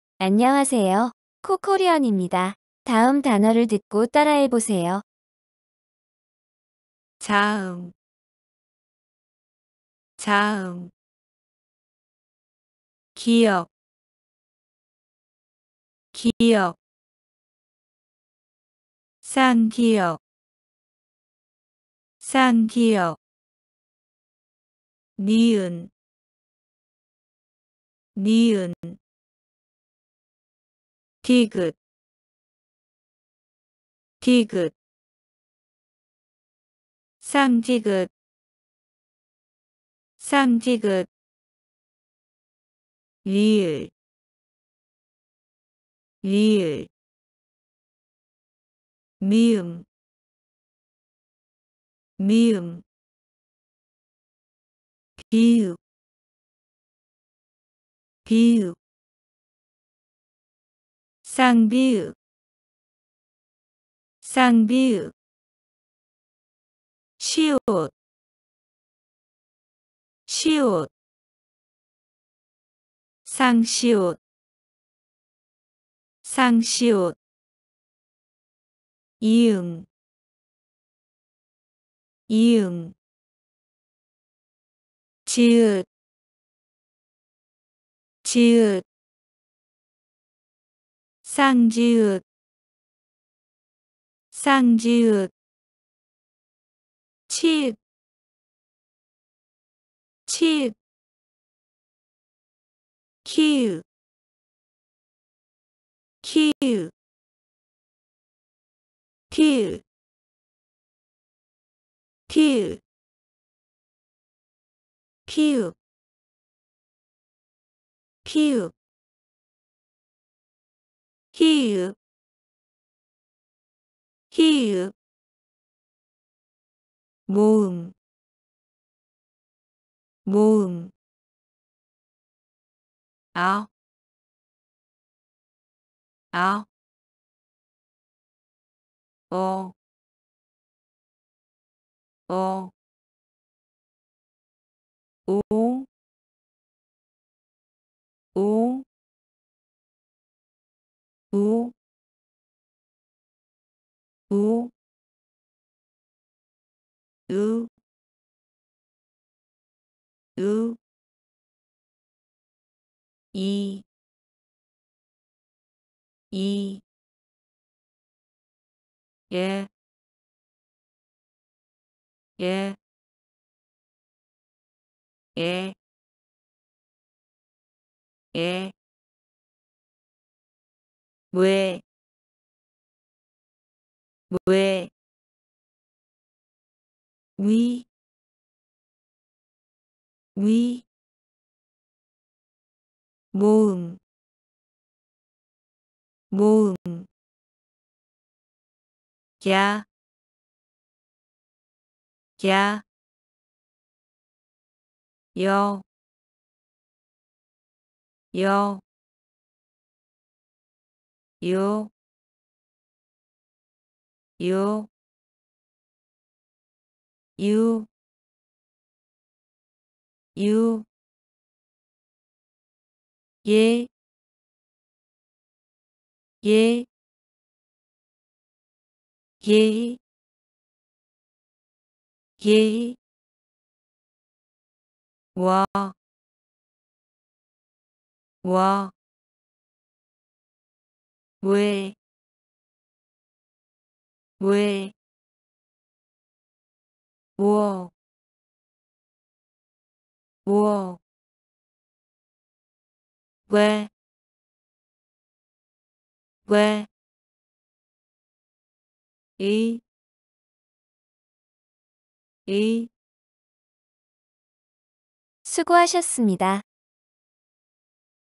[Korean Alphabet] Vowels & Consonants
Hi. It's CoKorean.
This video is for people who want to learn Korean.

The Korean Alphabet has 14 consonants and 21 vowels. Of these, 10 are basic vowels, and the remaining 11 are double vowels.

Listen, Read and Speak!